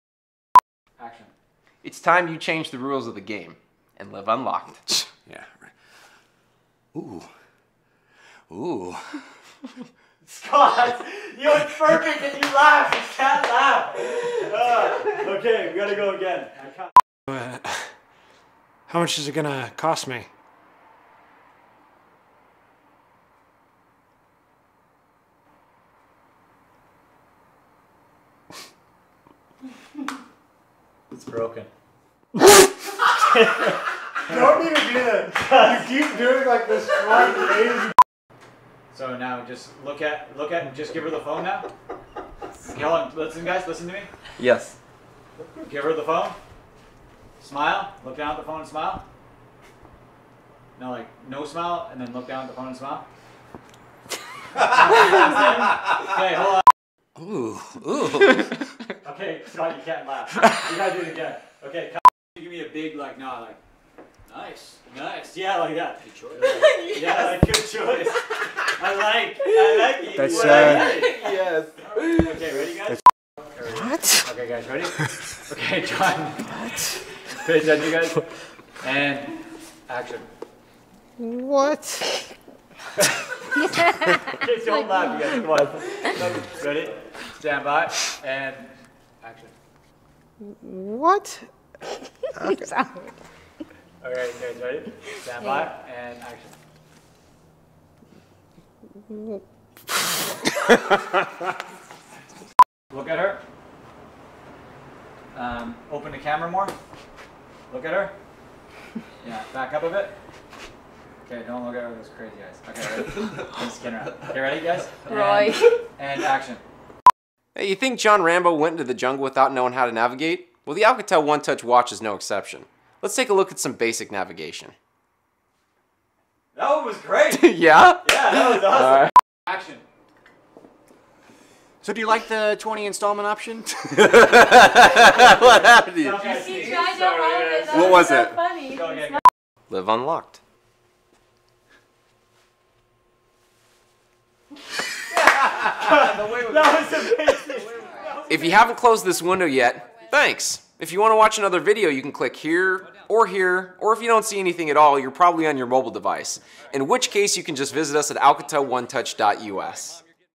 Live Unlocked Blooper Reel
Living unlocked means being able to laugh at yourself, have fun and live life without restrictions. Take a look at these behind the scenes outtakes from some of our favorite Live Unlocked videos.